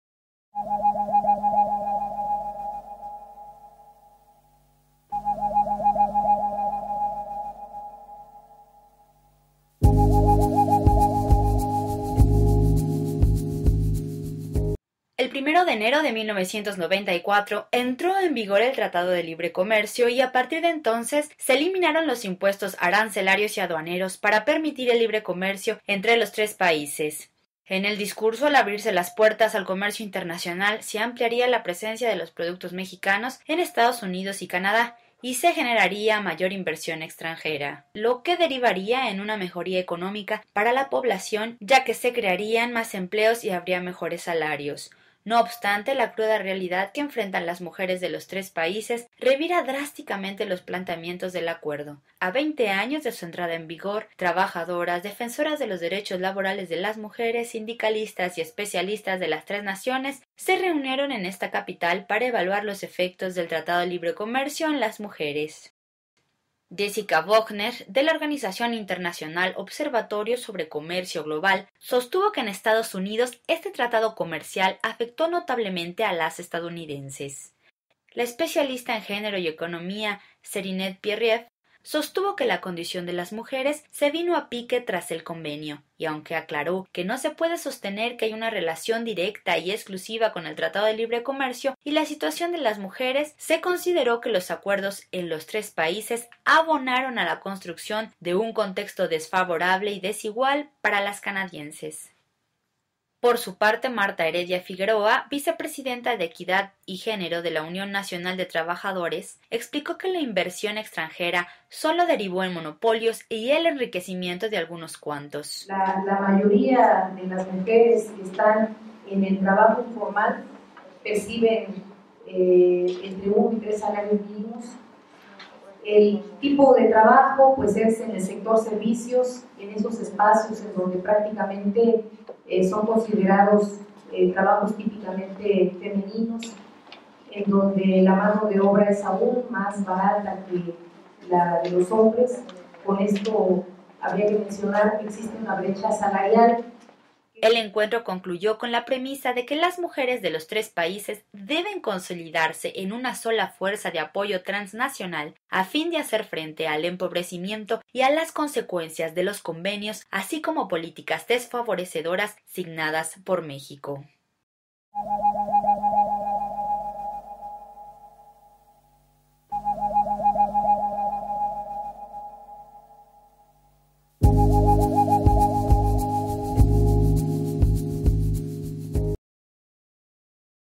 Devino en pesadilla para las mujeres el "sueño" del TLCAN
En 20 años, las promesas del Tratado de Libre Comercio para América del Norte (TLCAN) --acuerdo firmado entre México, Estados Unidos y Canadá-- sólo trajeron para las mujeres largas jornadas laborales a cambio de salarios mínimos, el empobrecimiento del campo y el desplazamiento forzado de miles de ellas.